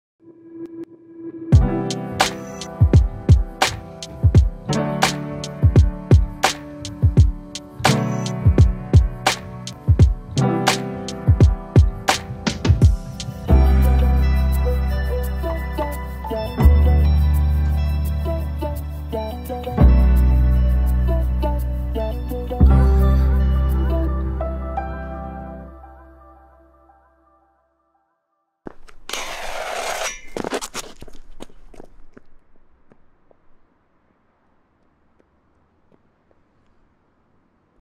Epic Dash Grindshoes [Product video]
The Epic Dash combines comfort with the ability to use the city as your playground. These high-quality Freeslide shoes have multiple lacing options for extra control and comfort, as well as a rugged construction for enhanced durability. Above all, the Epic Dash allows you to innovate and combine grinds with anything, or in other words, grab your Epic Dash and grind everything.